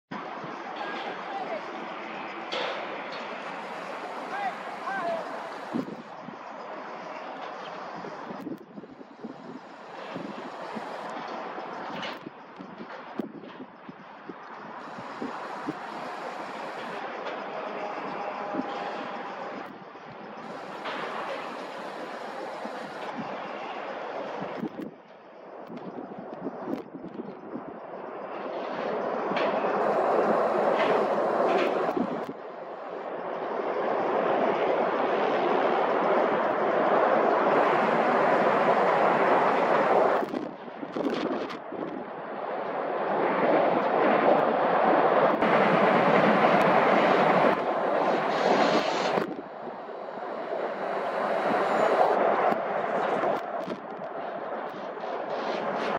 ニコンP1000の動画テスト
ズームテストです。手持ちなので安定していません。